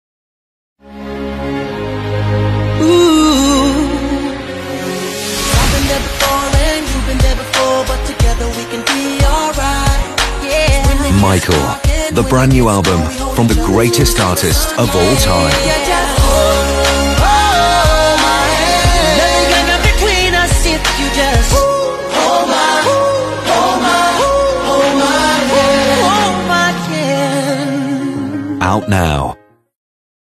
Michael Jackson the brand new Album
King of Music 💘🔝💝⭐
MJ’s eyes stare out of a partially obscured full moon that uses negative space to craft layers of eerie texture and meaning. Some shapes cut recognizable silhouettes – the descending crows, for example – while others suggest creepy, otherworldly creatures lurking in the shadows.